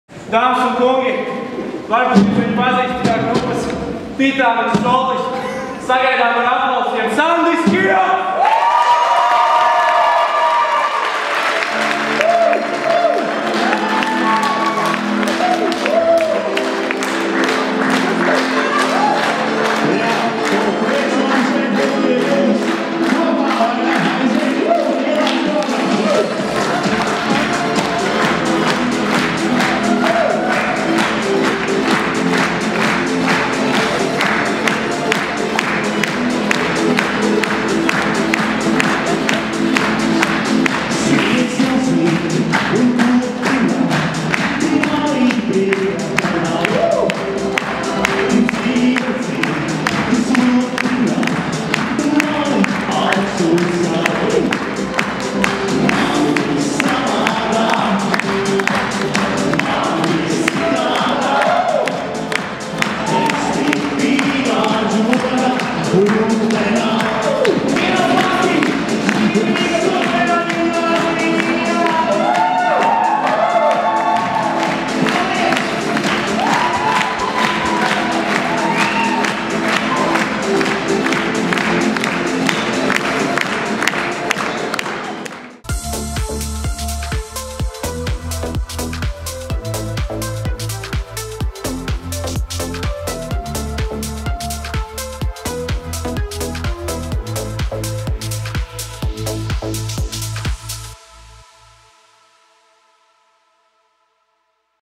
PĪLĀDŽOGA AR FANIEM RĪGAS OTRAJĀ ĢIMNĀZIJĀ ŽETONU VAKARĀ.
NIKNS PĪLĀDŽOGAS FANU UZVEDUMS AR MANU PIEDALĪŠANOS RĪGAS OTRAJĀ ĢIMNĀZIJĀ ŽETONU VAKARĀ.